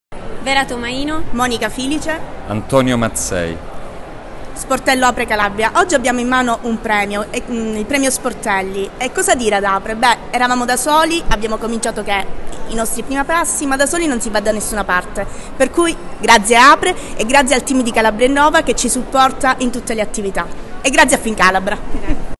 Tra i 17 Sportelli territoriali APRE the winner 2018 is... CalabriaInnova
Durante la Conferenza Annuale APRE 2018, CalabriaInnova è stato premiato come miglior Sportello Territoriale APRE 2017.